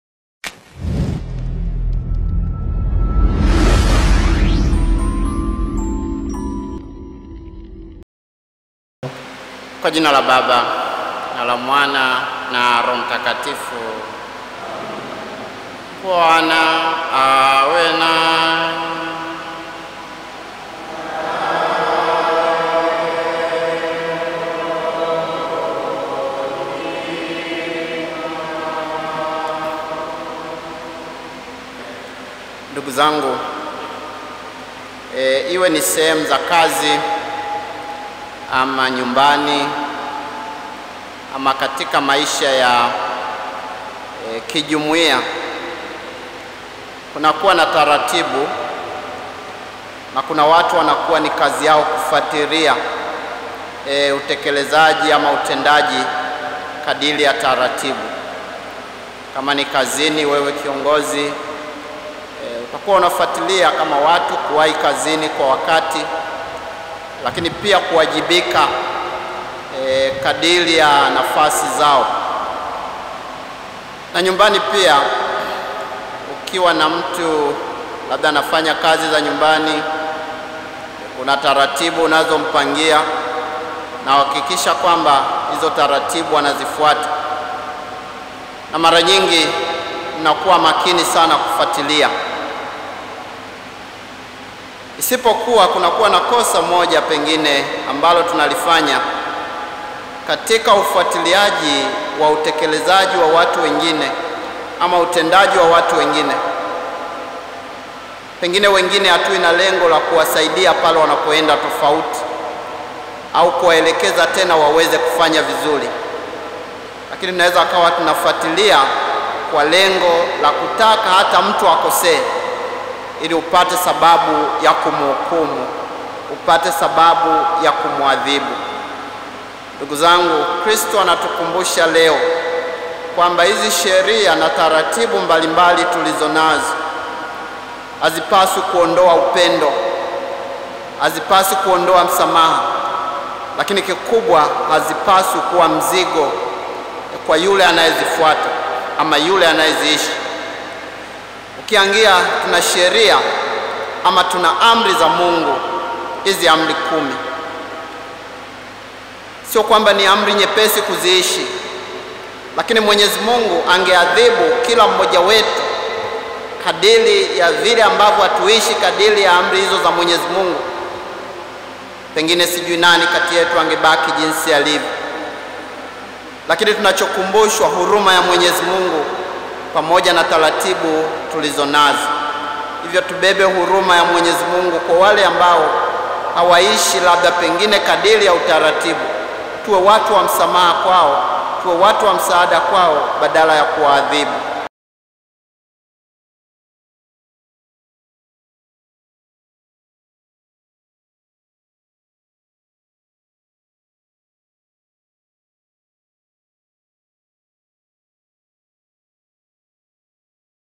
Tuwasamehe Wenye Makosa na si Kuwahukumu - Tafakari ya Neno la Mungu Leo Jumanne Tarehe 18/01/2022
Tuwasamehe Wenye Makosa na si Kuwahukumu - Tafakari ya Neno la Mungu Leo Jumanne Tarehe 18/01/2022 | Kumbukumbu ya Mt. Margareta wa Hungaria, Mtawa

𝕄𝕒𝕨𝕒𝕤𝕚𝕝𝕚𝕒𝕟𝕠:
JUGO MEDIA,